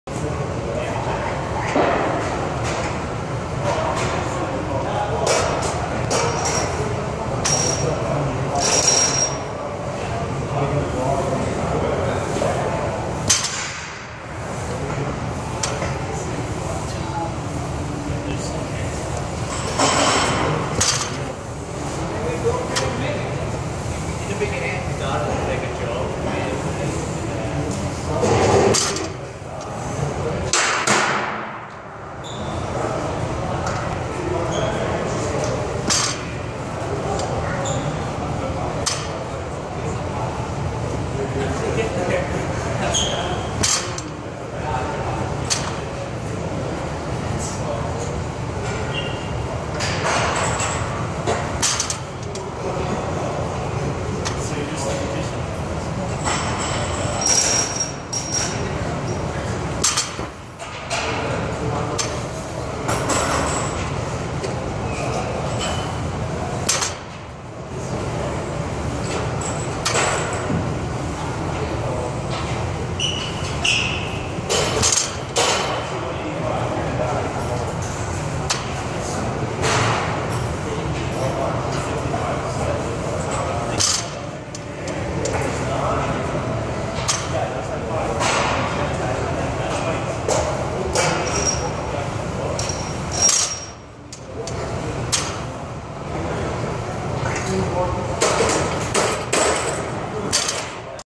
Deadlift Sunday - Before Coronavirus Gym Shutdown - PopularFitness.com
Got in my last deadlift workout on Sunday before the gym was temporarily shut down due to the coronavirus, COVID-19. The first time ever that a gym has been closed due to a virus, never thought this would happen in modern times.